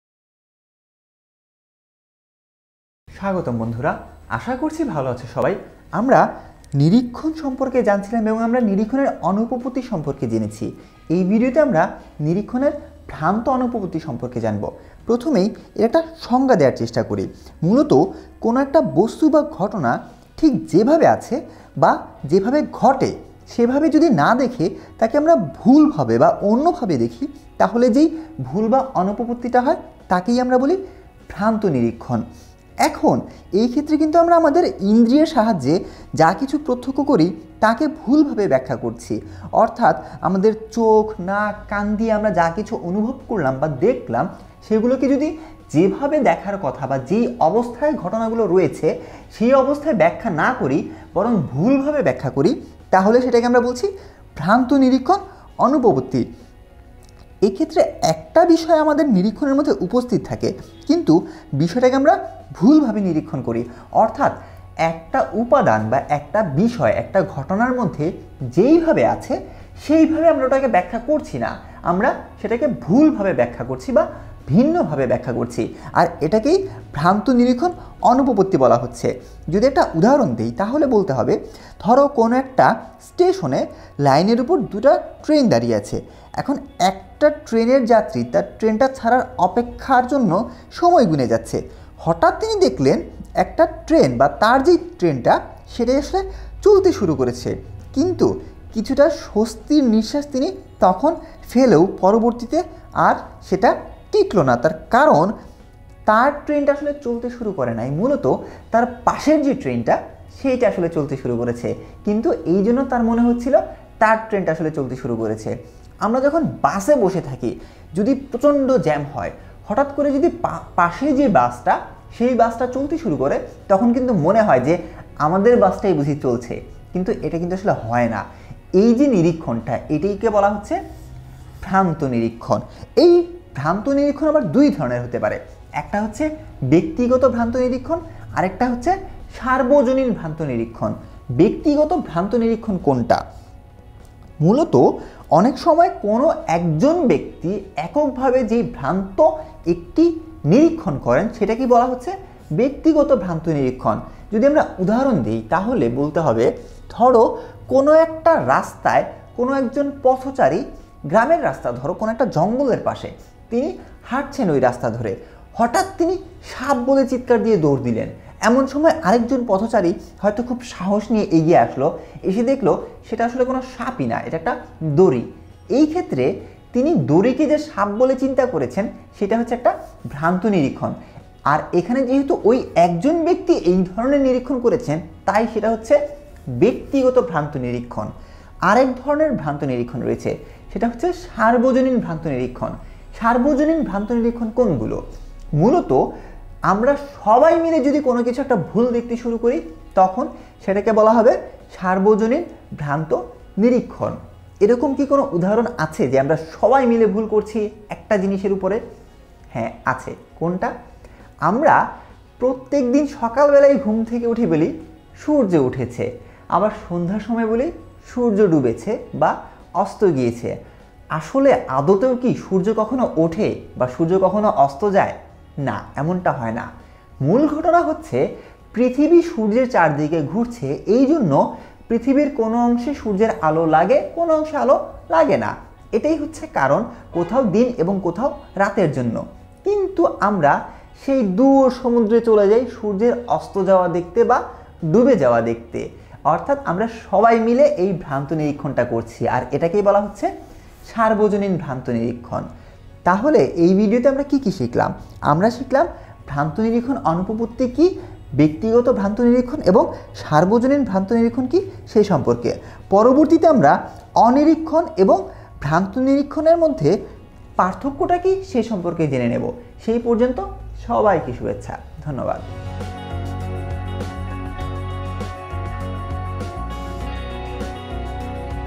০৭.২৯. অধ্যায় ৭ : আরোহ অনুমান ও আরোহ অনুমানের ভিত্তি - ভ্রান্ত নিরীক্ষণ অনুপপত্তি [HSC]
Series: Logic 1st Paper
Instructor Name:Maruf Mozammel